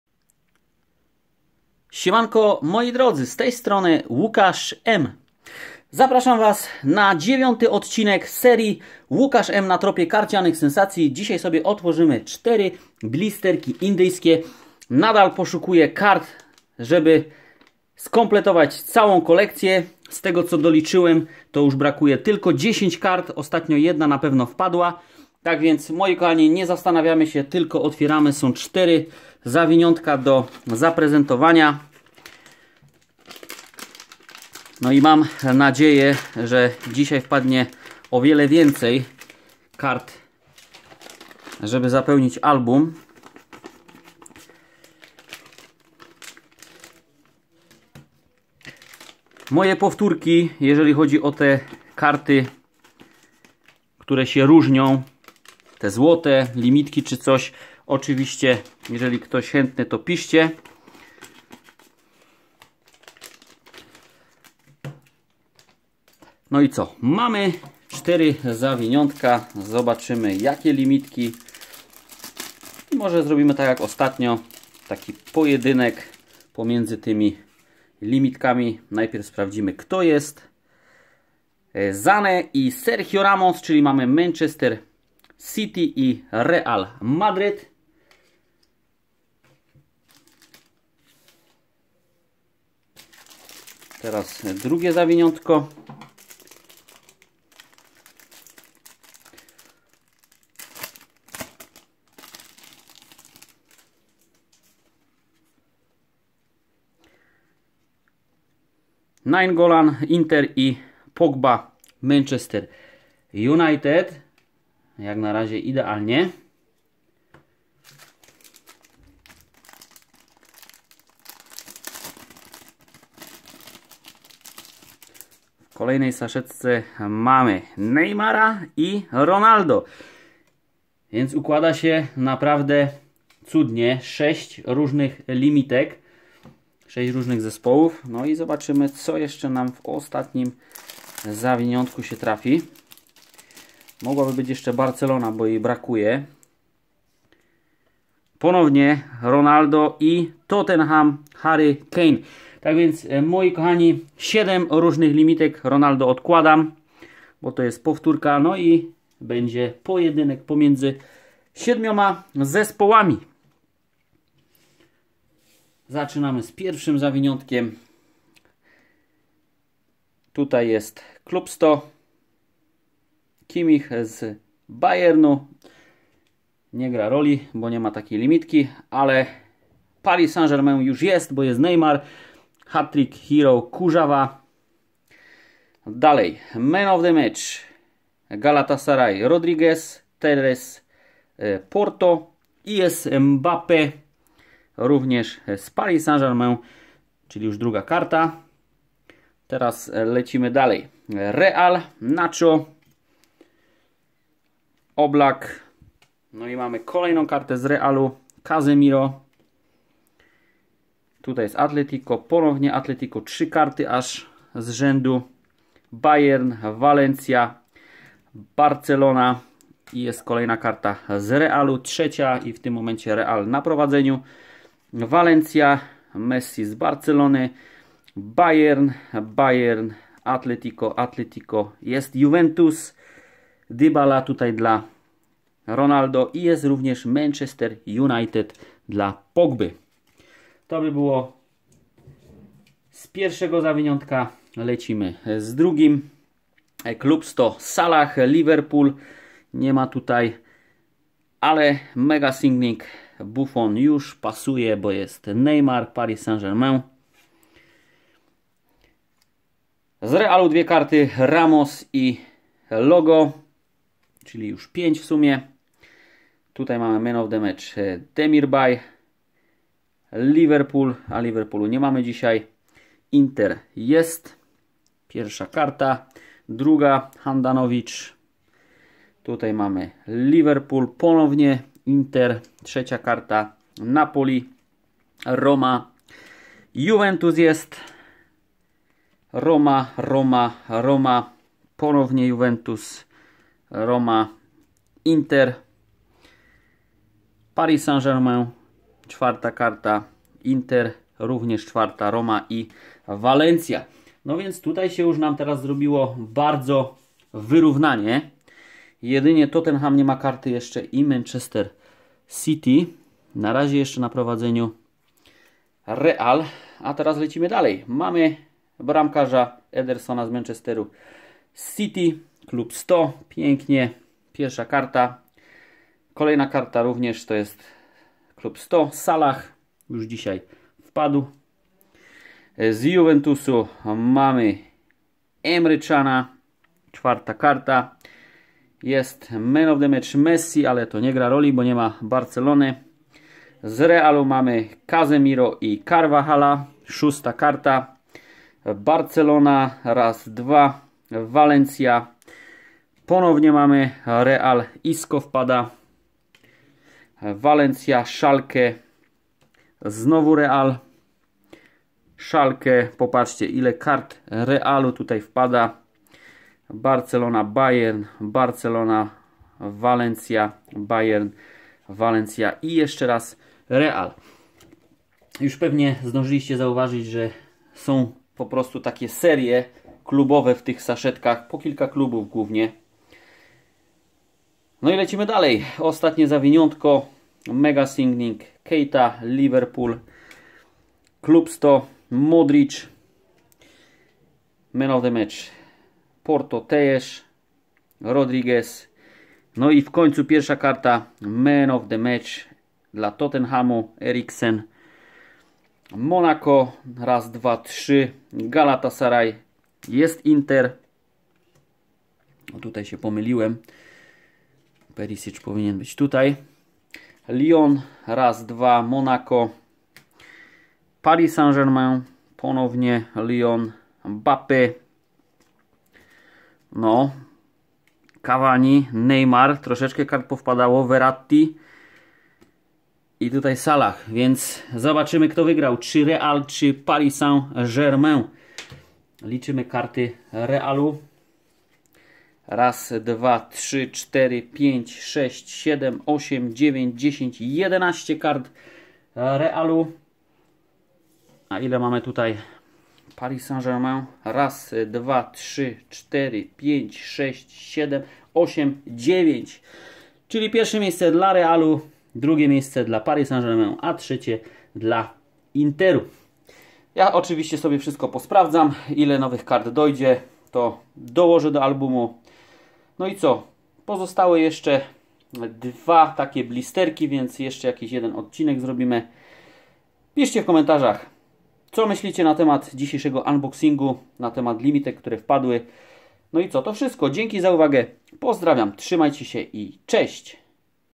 ŁUKASZ M. NA TROPIE KARCIANYCH SENSACJI ODC.10 - OTWIERAM 4 BLISTERKI Z INDII - CO WPADŁO???
Zapraszam na jeden z ostatnich filmów z kolekcją indyjską bo zostały mi już tylko 2 blisterki do otworzenia...Brakuje już niewiele ale ciężko będzie zebrać komplet.